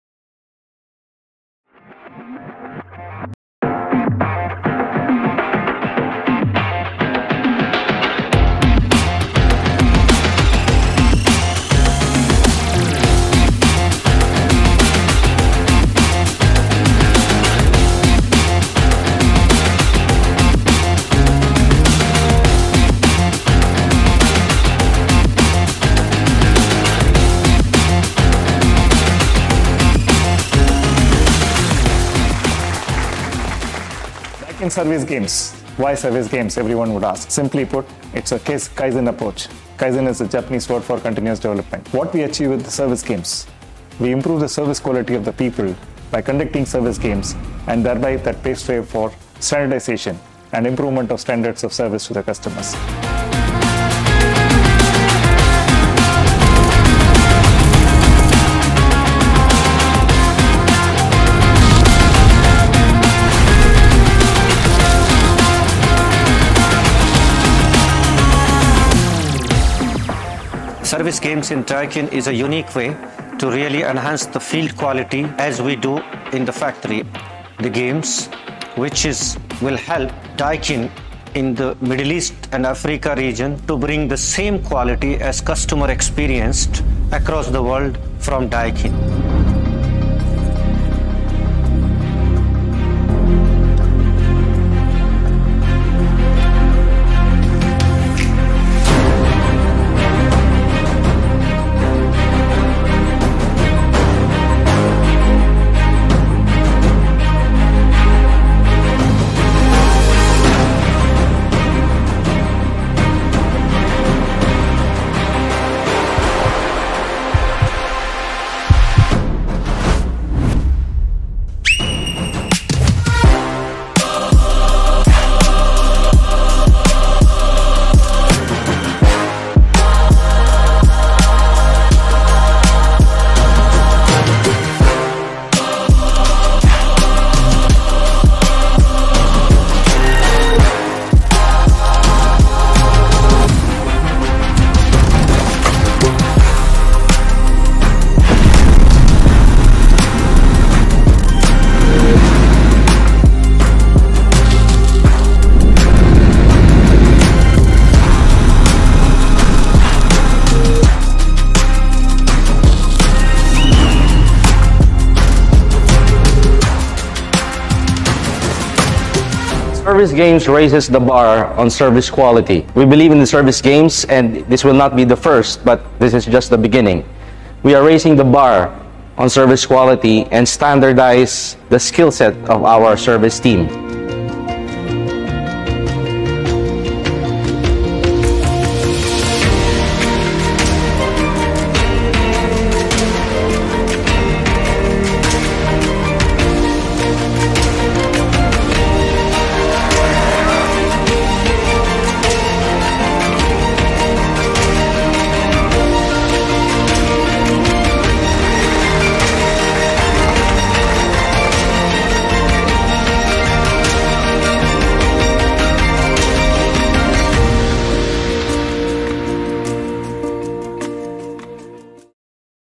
Daikin Service Games 2023
Daikin Middle East & Africa hosted their first ever Daikin Service Games in Dubai Headquarters from August 15 to 16, 2023.

Daikin Service Games is a unique way of standardizing the Service Quality across the group. It is aligned with Daikin’s value of providing prompt, reliable and customer friendly service worldwide. A commitment to providing exceptional Service Quality, by having well-trained service professionals available within customer’s reach.